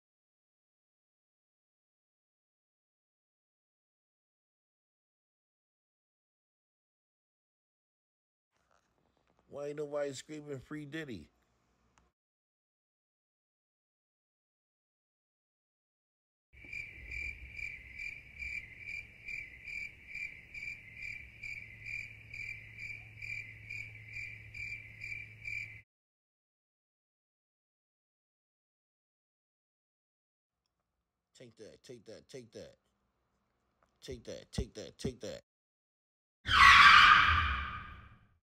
FREE Diddy?
Wondering why these shirts hasn’t surfaced yet ? And WILL they come out with some?